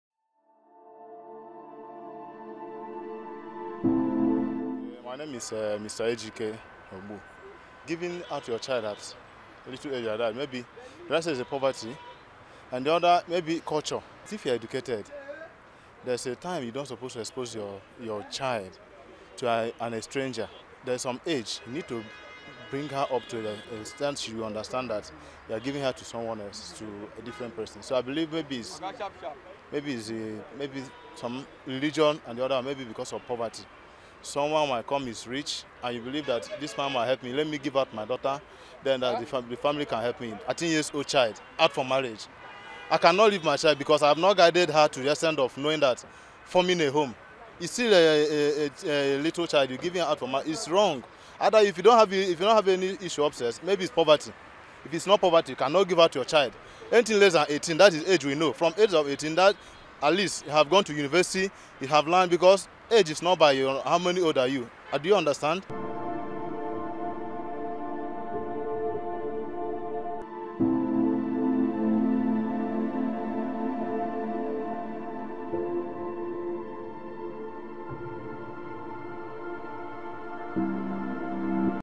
16 Days of Activism against Gender Based Violence.
The United Nations Children's Fund (UNICEF) has decried the number of out-of-school girls in the country, saying that over 60 percent of 10 million out-of-school children in Nigeria are girls.

Child marriage which is also a common practice in Nigeria rooted in traditional, economic, religious, and legal conditions that disproportionately affect girls and women, has been highlighted as a reason for the number of out school girls.

Nigeria's rates of child marriage are some of the highest on the African continent, and it is projected that Nigeria will have 29 million child brides by 2050.

There is need for us to slow down this curve and get girls back to school.

We spoke with a man and  father, he has this to say, and we hope every man will resolve to say no to Child Bride marriage and ultimately reduce the number of out of school children.

In commemoration of the 16days activism to end gender based violence against women and girls, let us UNiTE in our cause for more empowered women and girls.